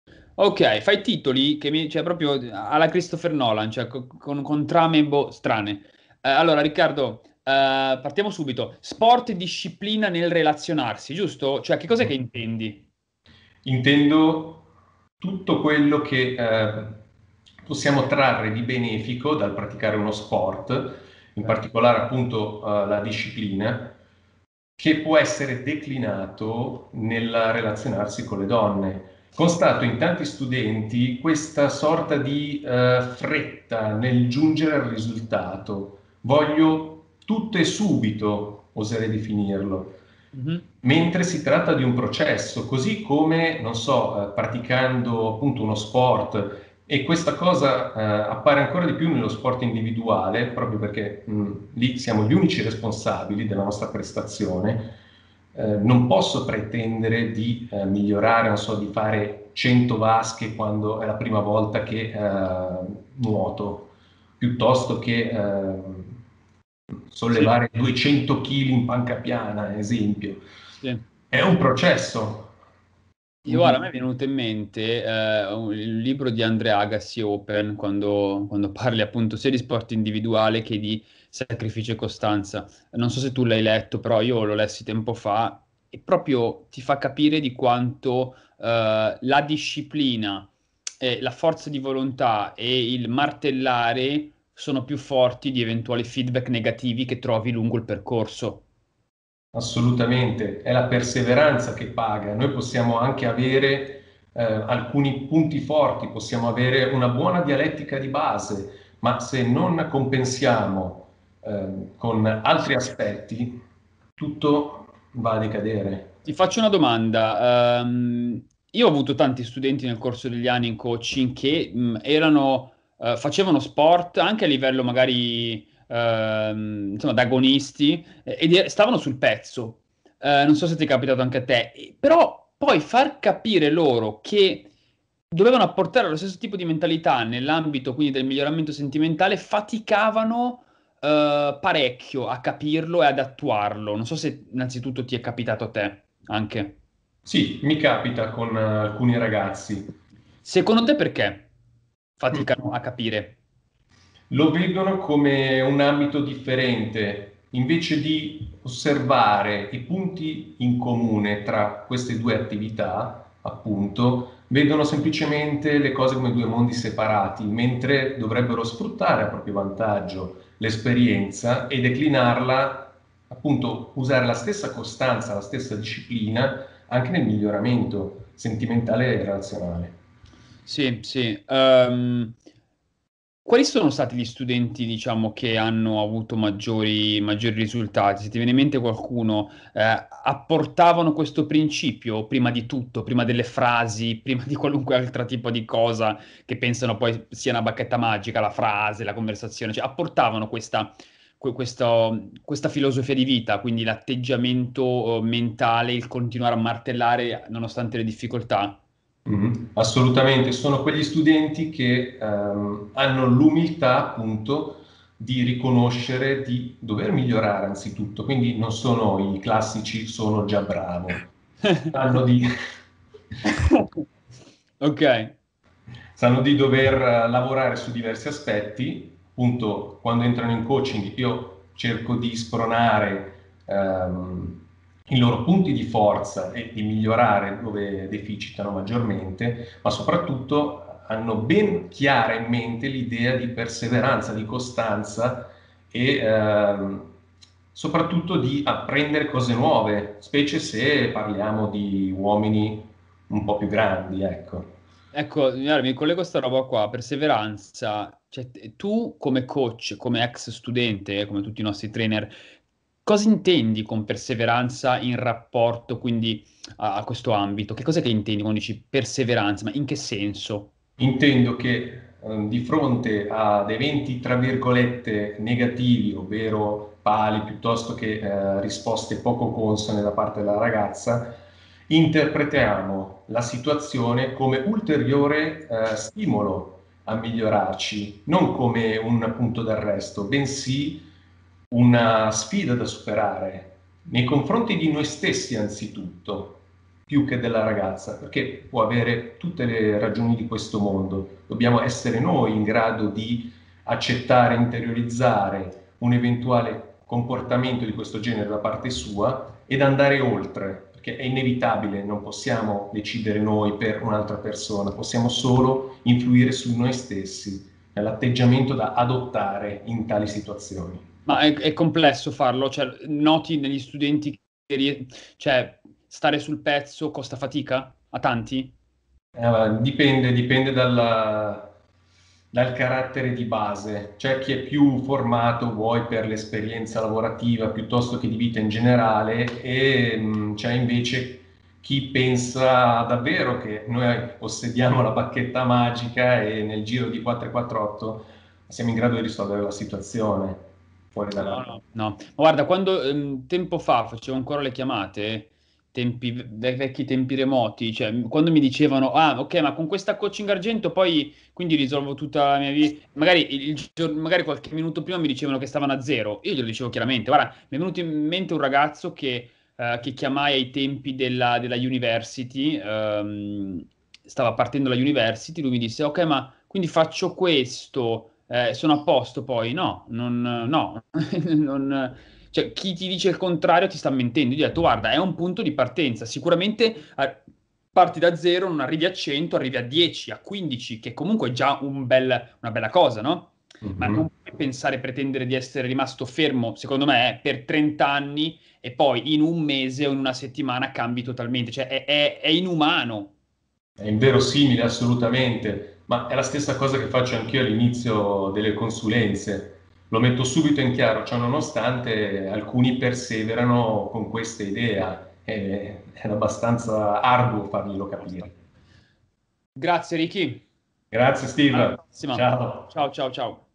La perseveranza nello sport e nel miglioramento sentimentale
Lunedì 4 Ottobre 2021 assisterai alla rivoluzione.
“Lavaggio del cervello” sarà l’evento più cattivo e sincero di sempre nel mondo del miglioramento sentimentale.

Autodisciplina in un percorso di miglioramento sentimentale

Esattamente come in ogni ambito, attività e sport la disciplina e perseveranza battono un talento innato.
Con le ragazze è la stessa identica cosa.